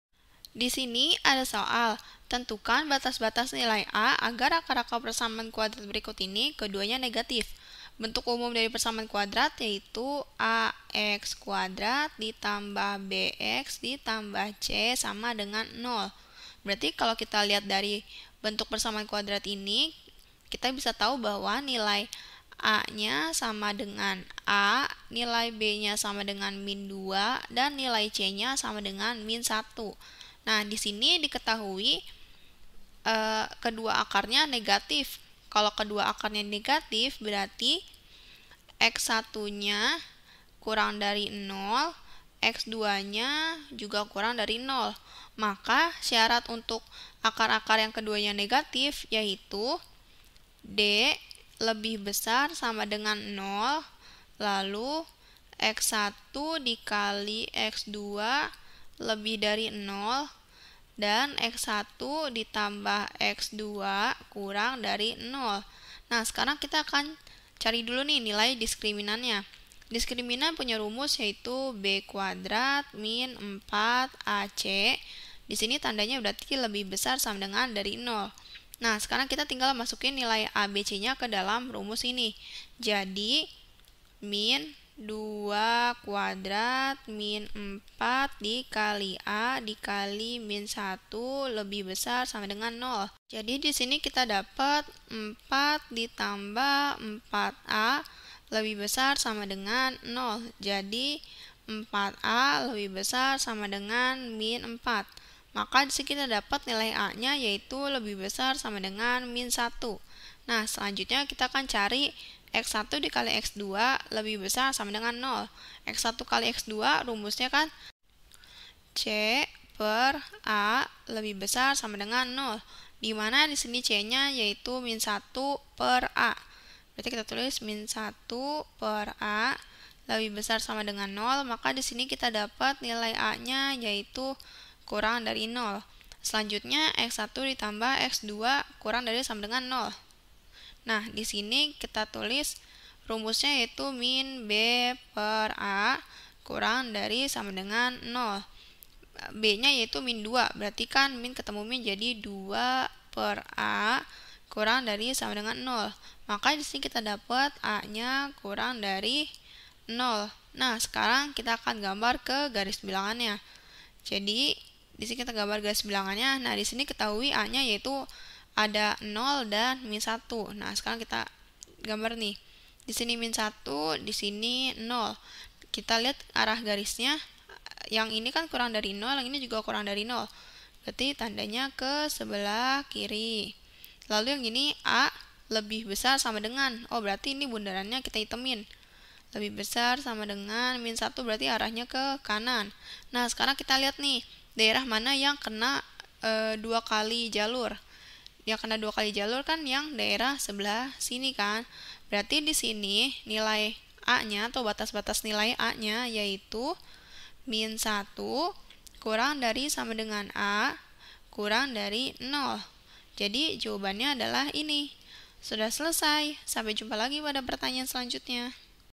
Tentukan batas-batas nilai a agar akar-akar persamaan kuadrat ax 2x - 1 =0 keduanya I negatif!
Topik atau Materi: Akar Persamaan Kuadrat - PERSAMAAN KUADRAT - ALJABAR - Matematika Kelas 9 SMP - K-13

Sumber Soal: Kumpulan Soal Matematika Wajib 1 kelas 10 K-13, ICE 2013 , IPEKA

00:00 : Bedah Soal
01:00 : Konsep, rumus dan pengertian Akar Persamaan Kuadrat
01:18 : Langkah penyelesaian soal
02:38 : Jawaban akhir
03:08 : Penutup